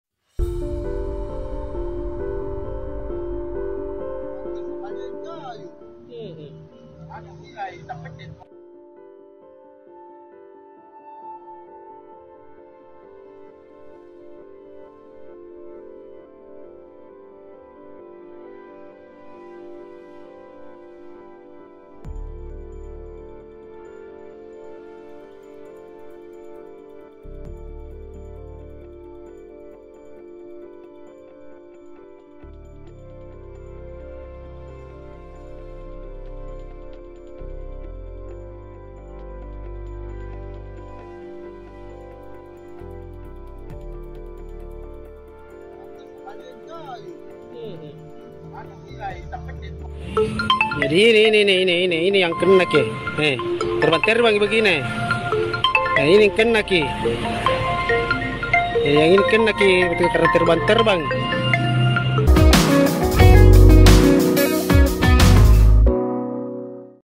Driver Ojol Terluka di Mata Akibat Spanduk Kampanye yang Menggantung Rendah
Seorang driver ojek online terkena alat peraga kampanye di bagian mata saat melintas di Jalan Abdesir, Kota Makassar, pada Rabu (9/10/2024).

Dalam video yang beredar, terlihat driver tersebut menahan rasa sakit di area mata setelah terkena spanduk yang tergantung rendah di tepi jalan.

Spanduk tersebut diduga menggantung terlalu rendah, sehingga mengganggu pengguna jalan yang melintas.
Video terkini dan terbaru..